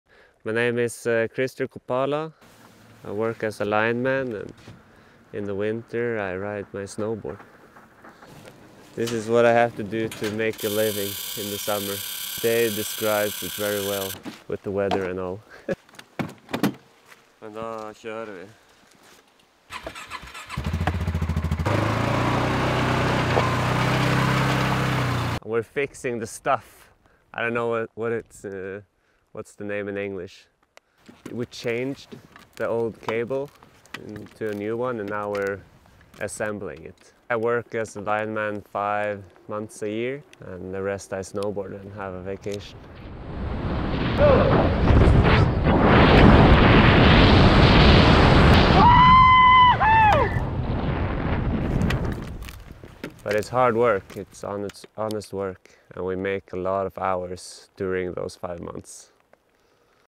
Lineman | Krister Kopala
Rab athlete Krister Kopala is no stranger to hard work.

Krister discusses the balance between his life as a big mountain snowboarder throughout the winter season, and his job building power lines in Norway’s mountains during the summer months.

“Mountains give me an overwhelming feeling of being small and not having control. I love going out there and working like I do with my job.

For example, figuring it out as you go, checking out the snow, finding anchor points, working out if you’re going to rapel.

I work as a lineman, building power lines during the summer so that I can go snowboarding in the winter.

It's hard work. I’m outside all the time in all types of weather. It doesn’t matter if it’s stormy, a thunderstorm, raining, or freezing.

You learn how to manage the cold and these conditions.

This keeps you in shape for the winter. It’s physically hard work, carrying heavy gear, walking a lot, with heavy backpacks.”